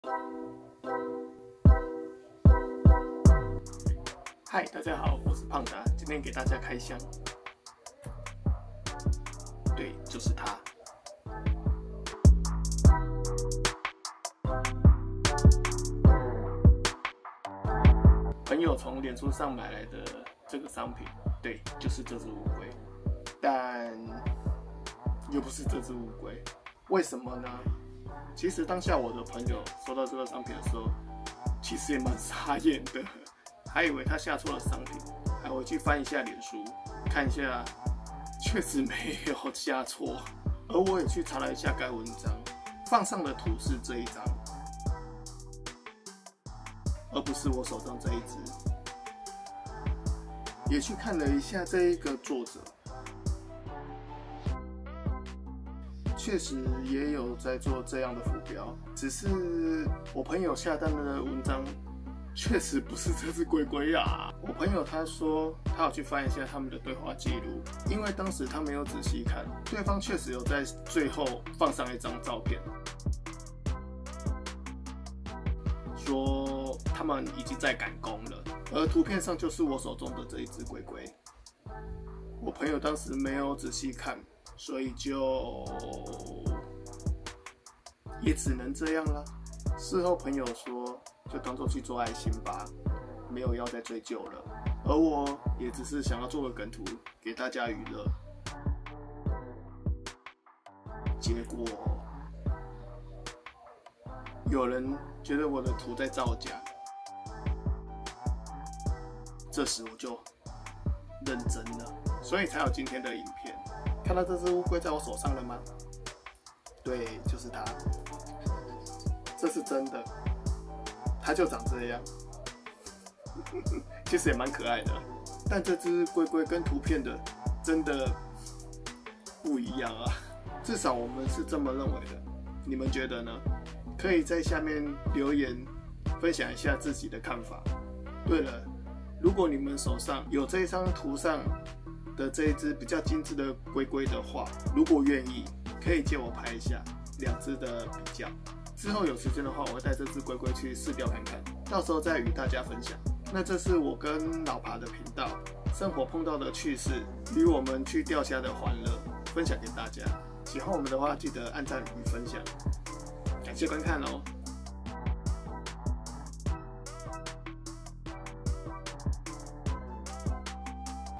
CC字幕 新年第一部 ! [開箱] 你不是我認識的龜龜 XD 大家新年快樂 !
請開啟CC字幕才看的到文字喔 !

朋友買了一個應該是圖片上的商品
結果出現的是這樣的商品...